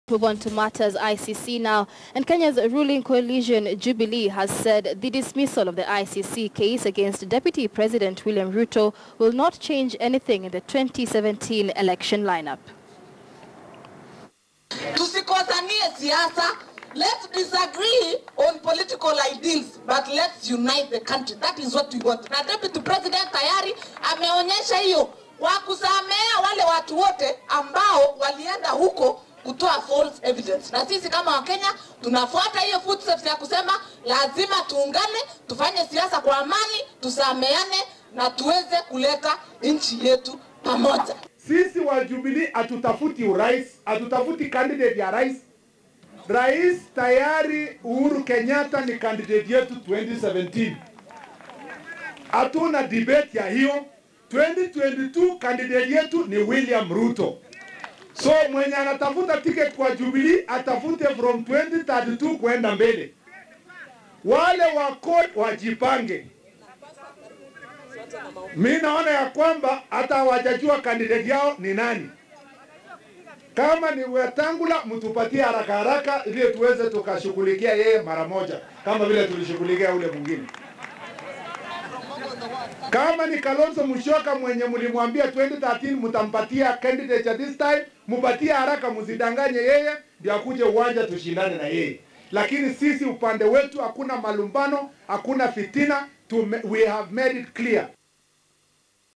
ICC ruling won’t affect Jubilee’s 2017 election line-up, a section of leaders say
Still on ICC matters, Kenya’s ruling coalition Jubilee has said the dismissal of the ICC case against deputy president William Ruto would not change anything in the 2017 election line-up.